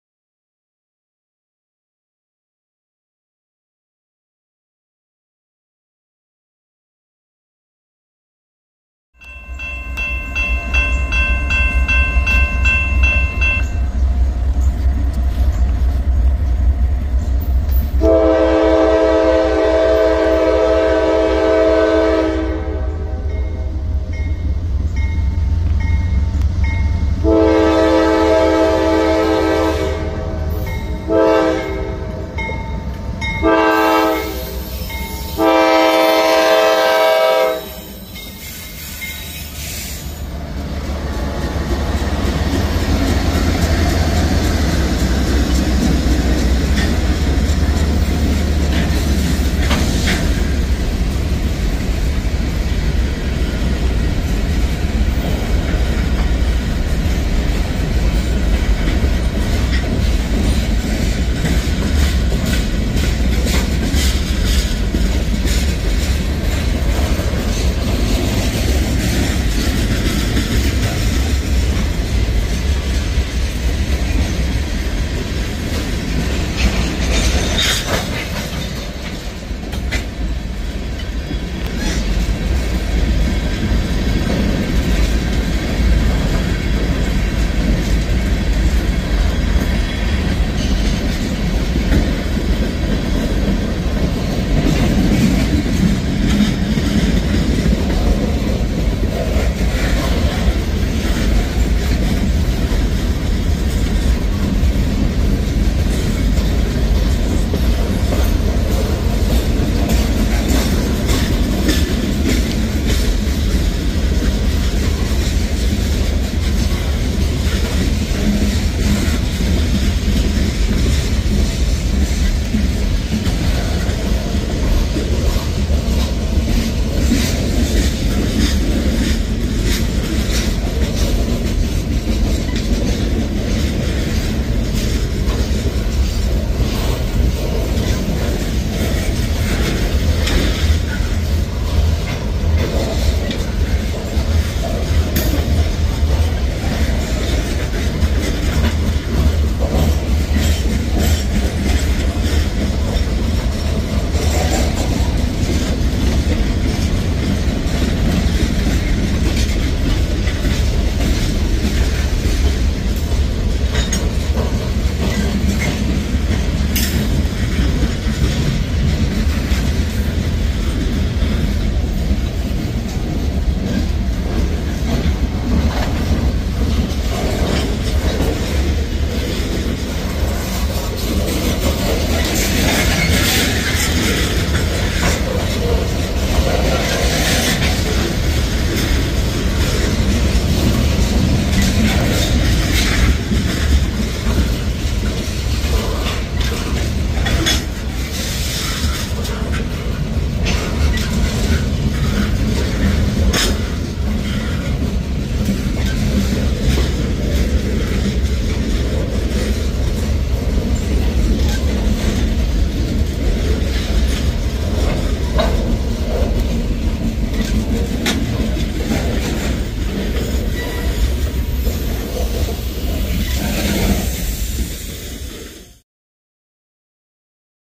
Norfolk Southern 192: Freight Journey Through Augusta's Twilight
Early Arrival of Norfolk Southern 192 Mixed Freight with a crew change before a Nightly department Northbound for Columbia Sc

0:00 Intro
0:10 NS 192
3:45 Ending